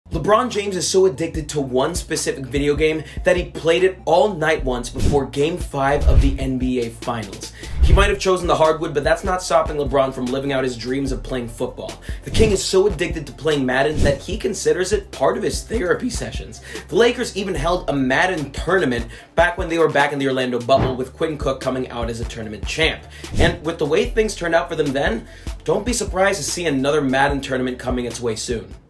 LeBron James’ Video Game ADDICTION 😎 | Clutch
Los Angeles Lakers superstar LeBron James is unarguably one of the best players to ever play the game. But did you know he has an addiction? The Space Jam 2 star and billionaire is addicted to Madden!